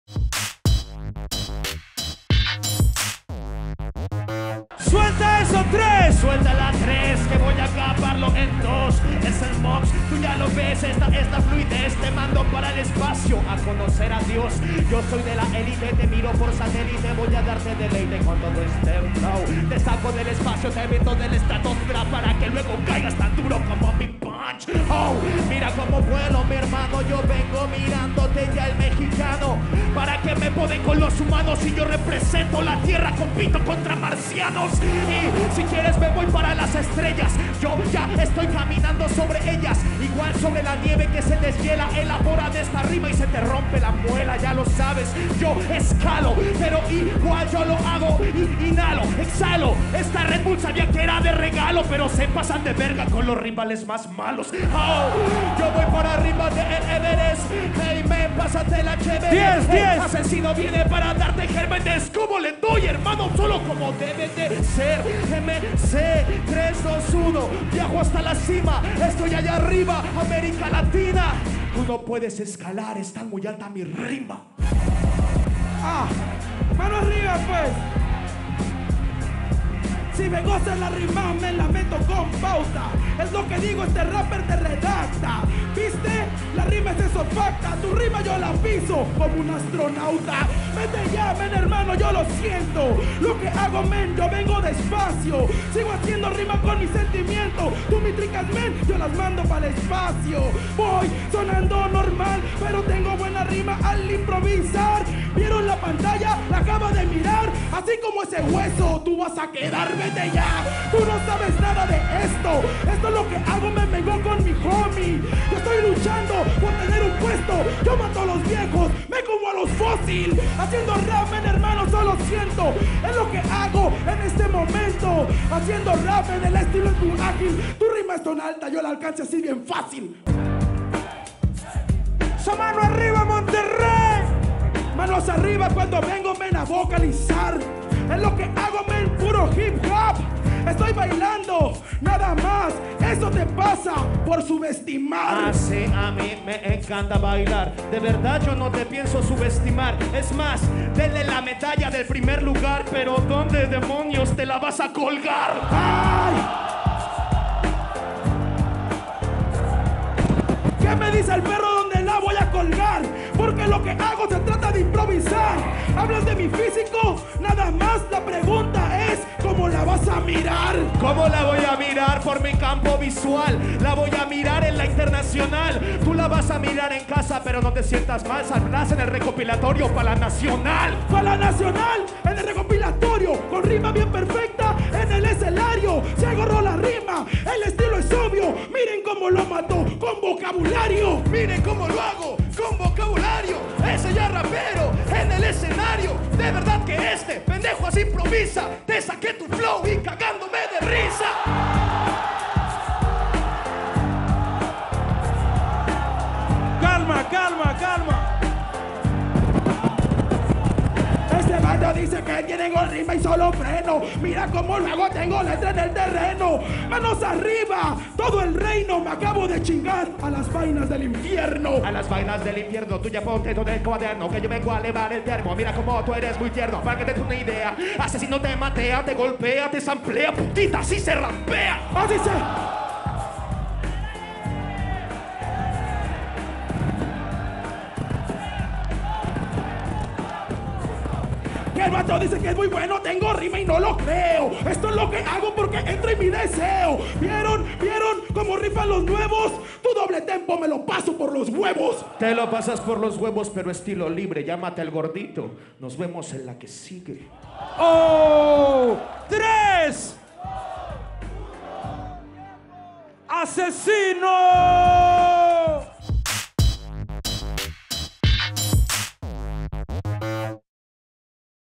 ACZINO vs MOKS - Cuartos: Final Nacional México 2017 - Red Bull Batalla de los Gallos
Revive la batalla en la que Aczino se enfrenta a Moks en la final de México 2017.